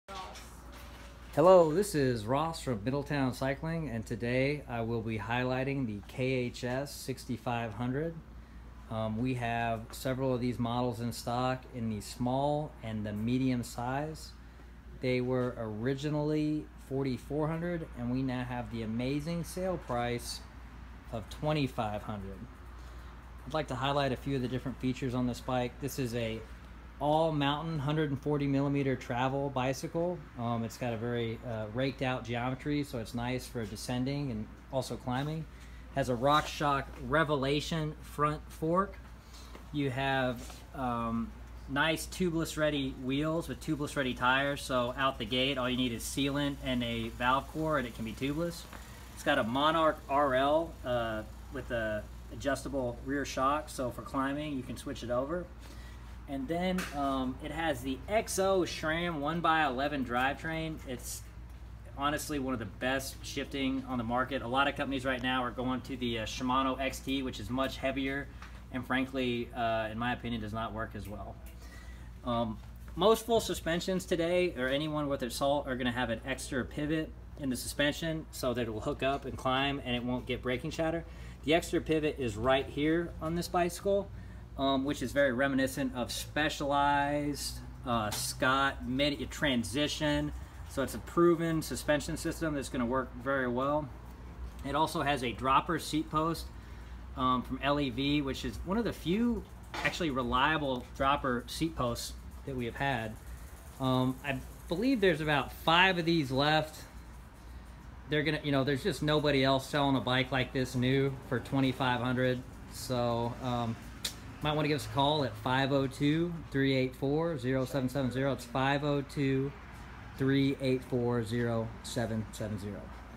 Middletown Cycling KHS 6500 Demo Video
Middletown Cycling review of KHS SIXFIFTY 6500 Full Suspension Mountain Bike